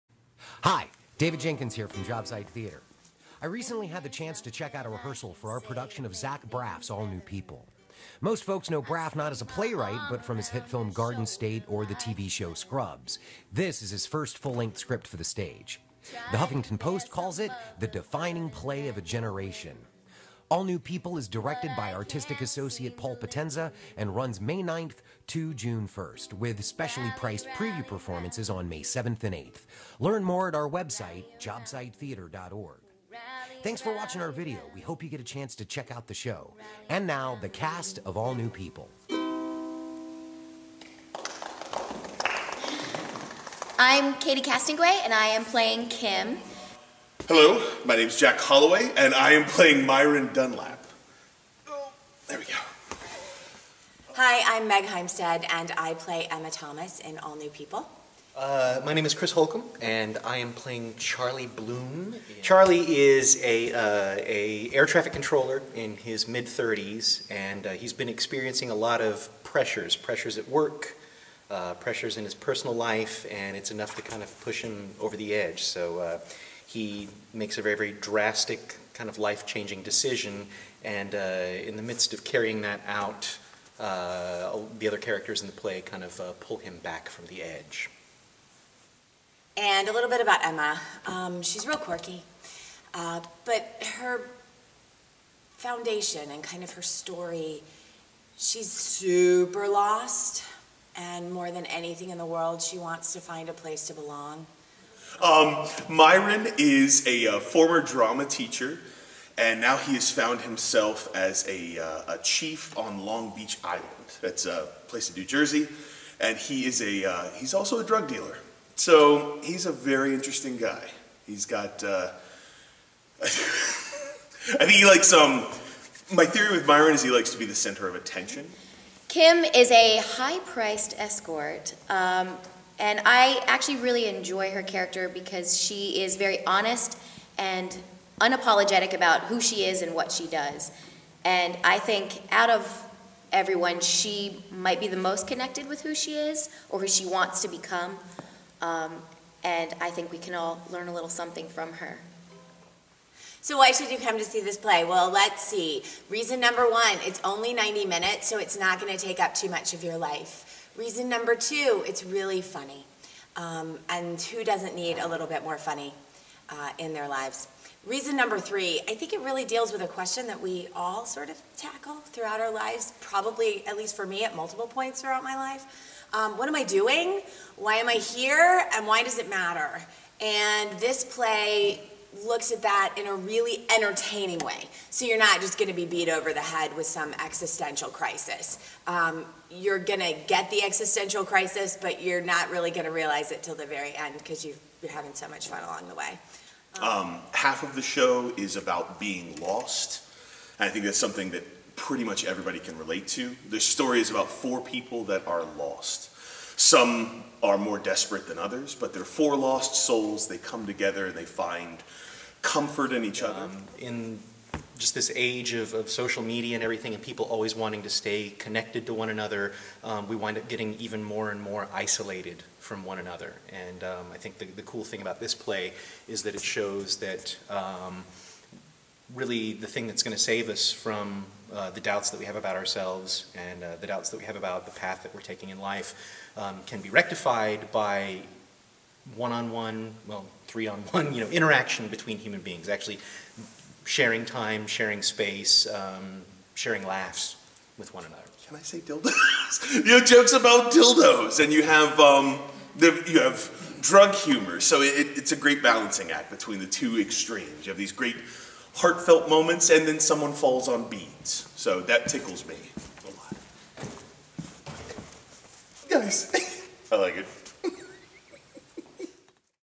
Jobsite Theater | All New People cast interviews
Cast interviews for Jobsite's All New People, which plays the Straz Center in downtown Tampa May 7 - June 1, 2014.